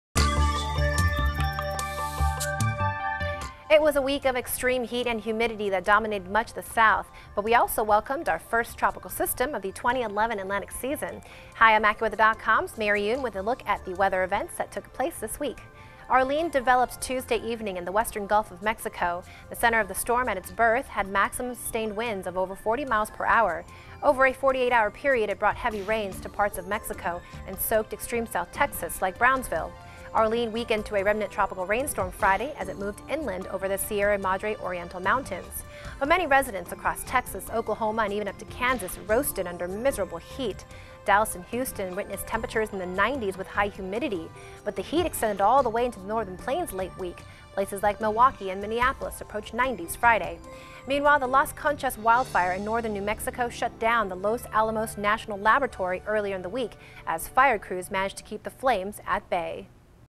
Extreme Weather - July 3, 2011: Week in Review - Extreme Heat and First Tropical Storm
The birth of Arlene kicked off the 2011 Atlantic hurricane season. Meanwhile, extreme heat affected much of the South. Mary Yoon takes a look back at the week.